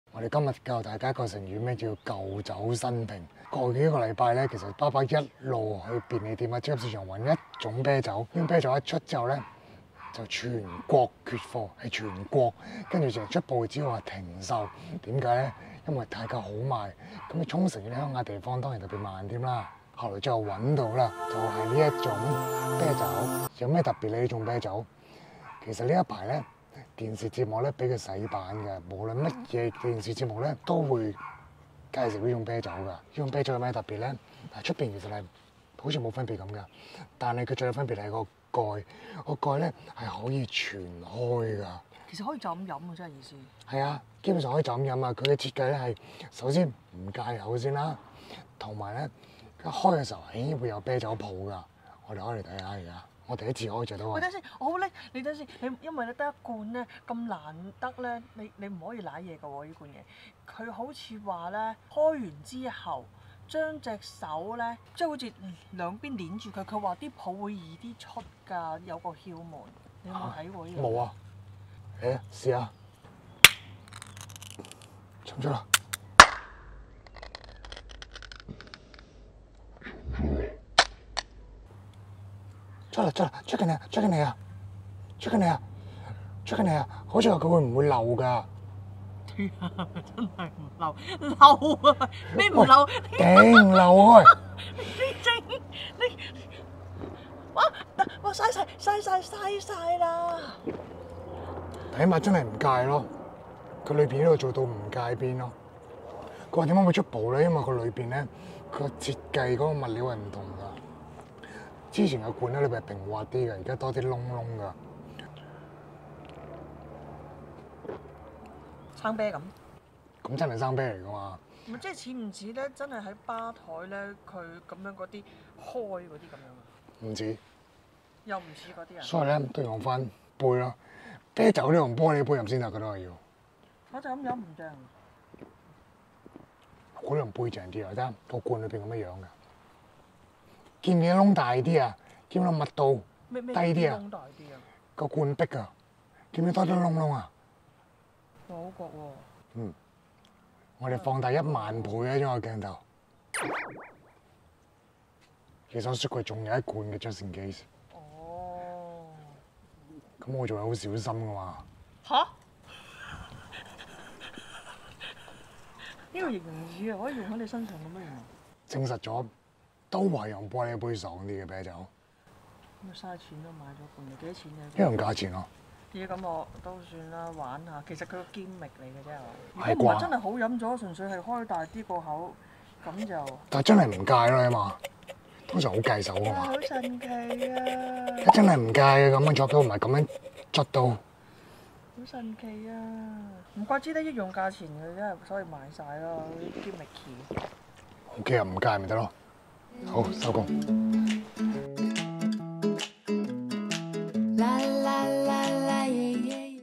舊酒新瓶 | 全日本缺貨 的 啤酒 開箱(中字）
日本朝日啤酒Asahi 在四月初推出了一罐新啤酒，出售兩日之後全國停售。不是因為回收，是因為完全供不應求！一瞬間俾人搜購一通。

啤酒本身完全沒有分別，但是啤酒罐重新設計，可以說是一個劃時代的創新。就是這種設計，重寫了這種啤酒的需求。

終於買到了！馬上開箱，告訴大家有什麼分別！